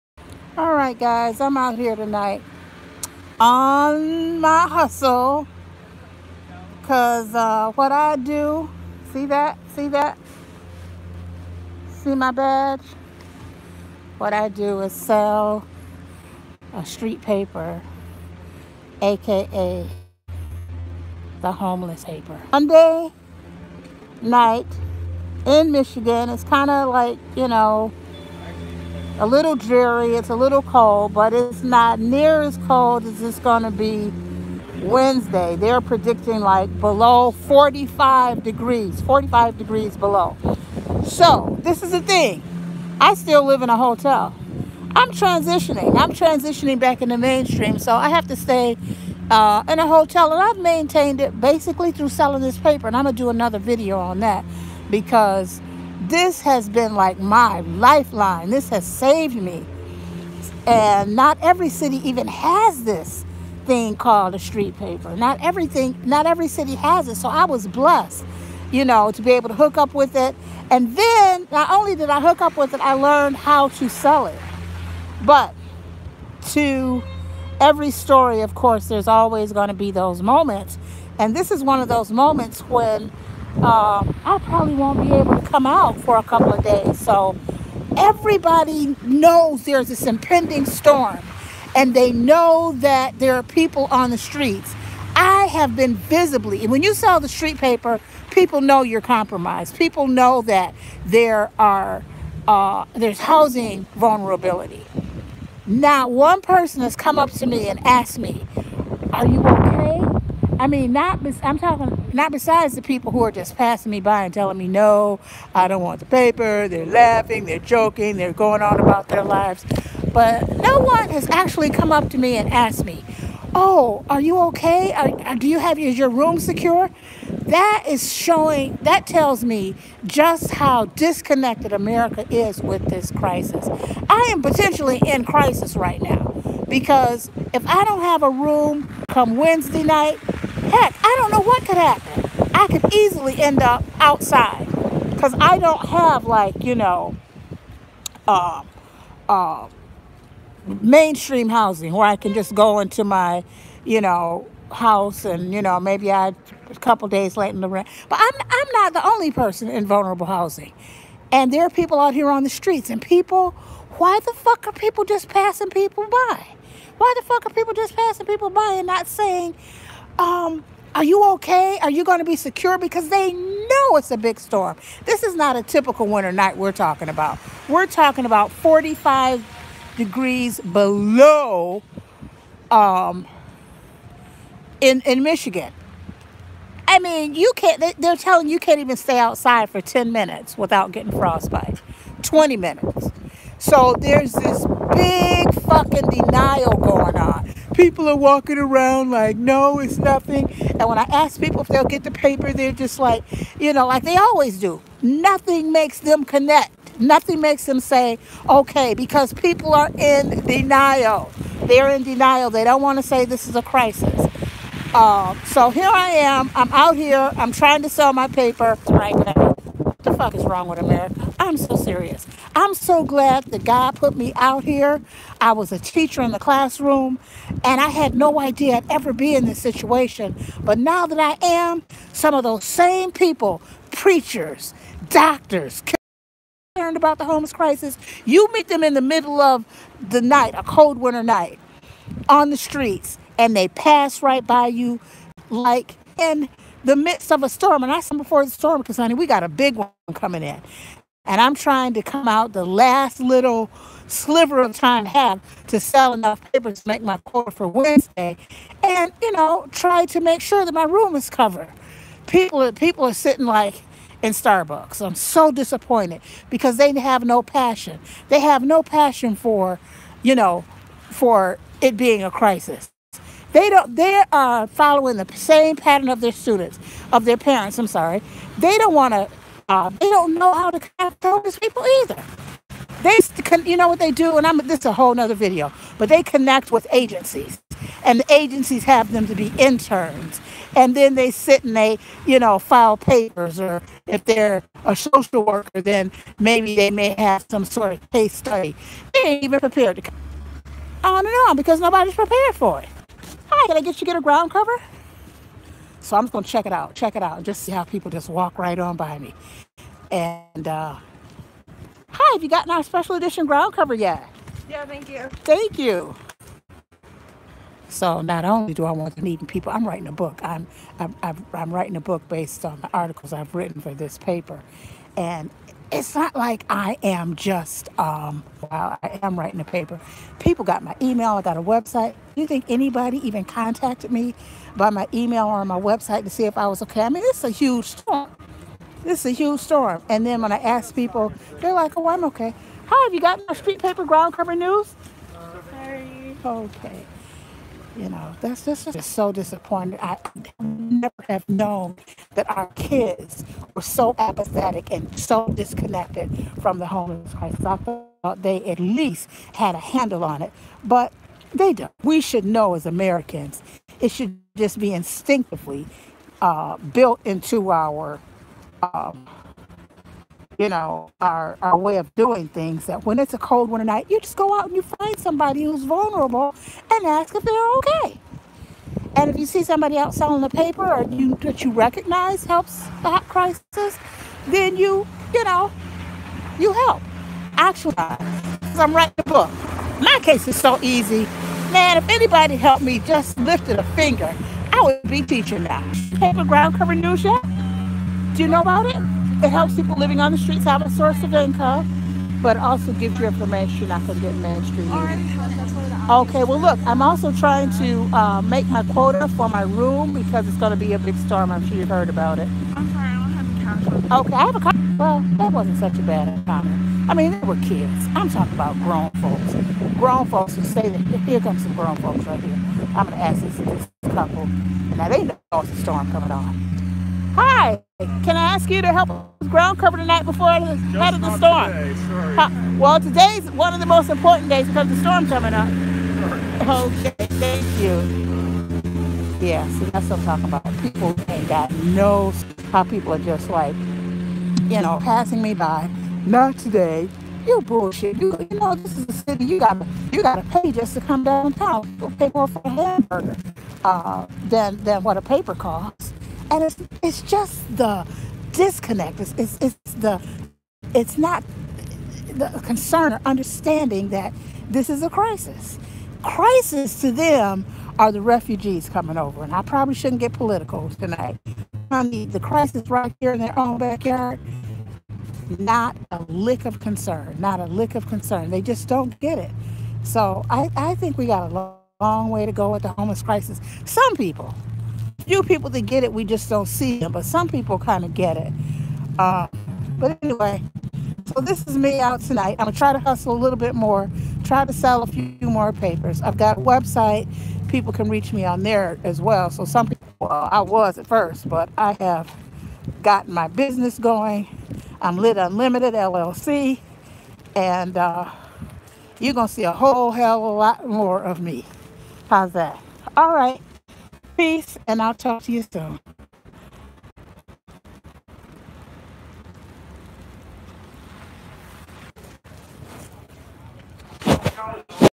Street paper sales before polar vortex
I often talk about how living in a liberal environment has not really affected some of the crucial areas of my homeless experience. This video was made shortly before the Polar Vortex when I was selling my street paper in hopes to make enough to keep my hotel room.
I had to deal with two problems. First, standing in the cold on one of the coldest nights of the decade to sell the papers. And, secondly, the fear of not selling enough papers to keep my hotel room.
If you think the hotel would have pity on me because it was a Polar Vortex, you are wrong. I almost lost my room during the pandemic when businesses shut down. Hotels did not have to adhere to the moratorium.
The street paper is not panhandling. It is designed to give a business opportunity for those who live on the streets. We sell it and write for it. It is a carefully edited publication and we undergo an orientation to sell it. But, during this polar vortex and now the pandemic., no one rushed out to buy one. It was just business as usual.